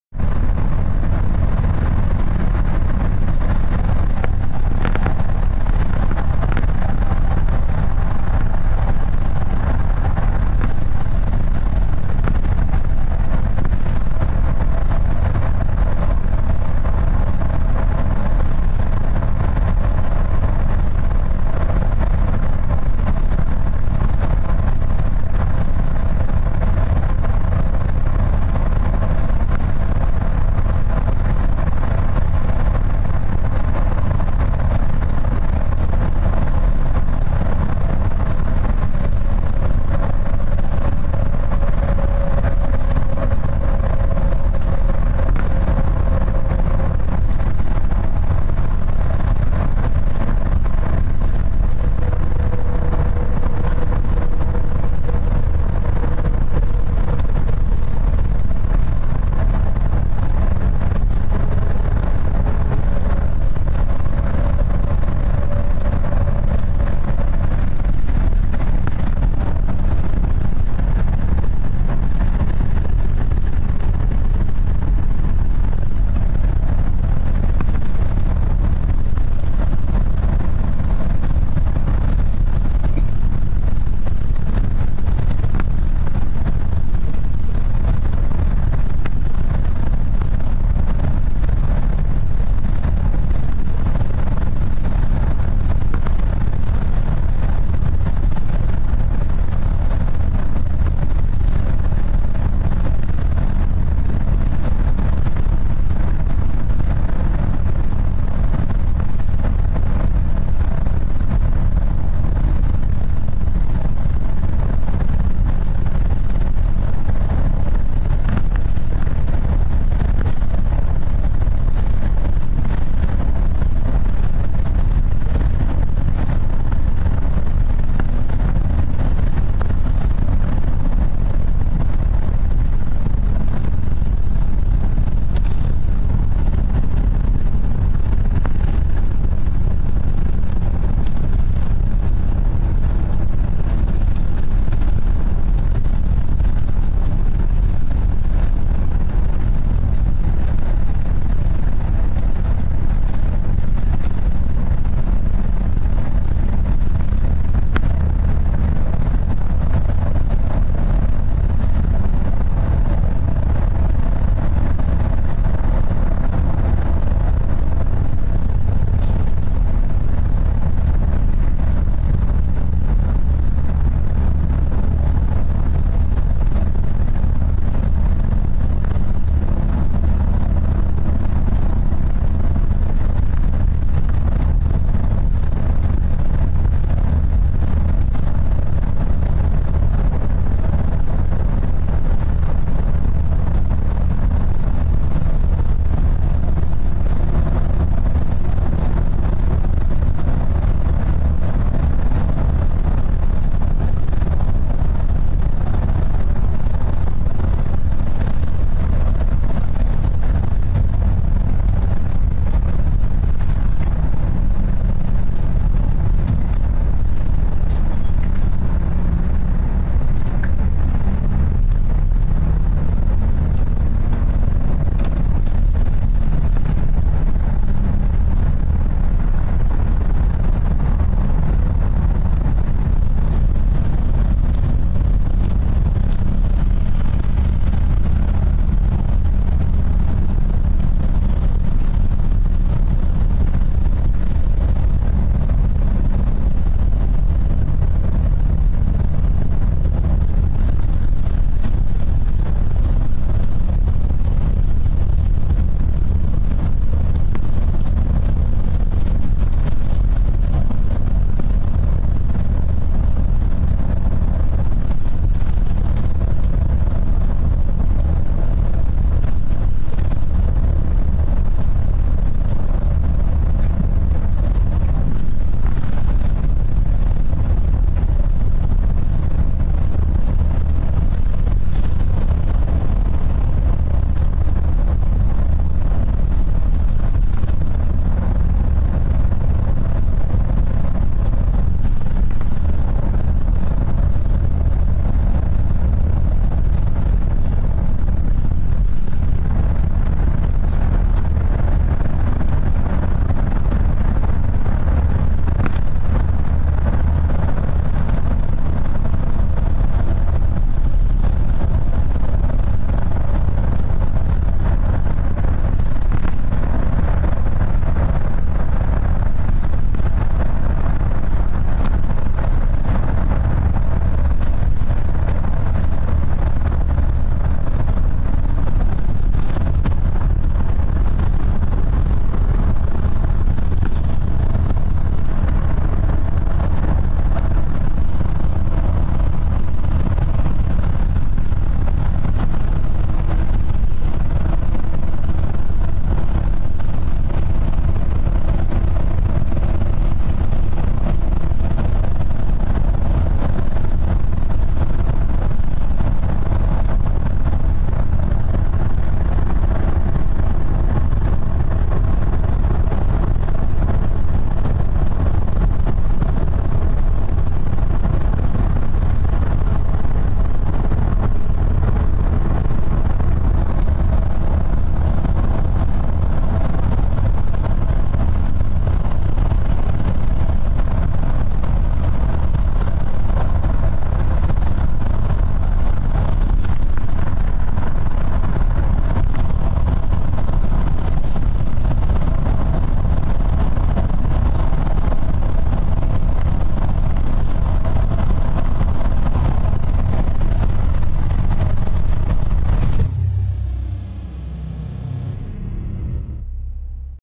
Ride on Sound Transit Line 586 thru Seattle
Ride on Sound Transit line 586 express from Tacoma to U-District, this is thru Downtown Seattle on I-5.